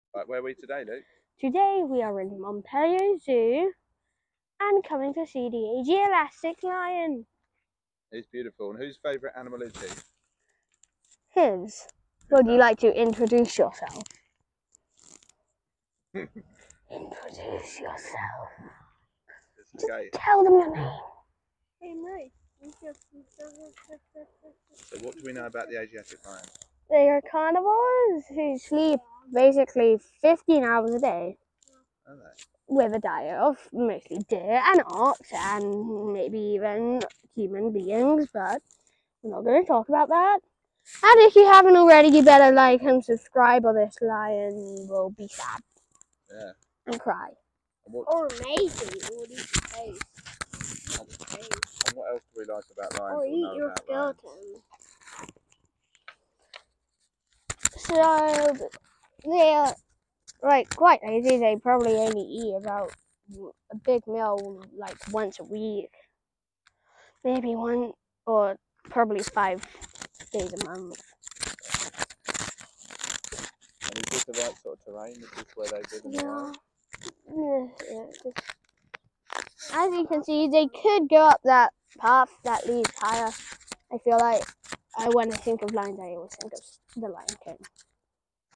Montpellier Zoo - Asiatic Lion
Z00Boy looks at lions with his fat brother Rafebacca at Montpellier zoo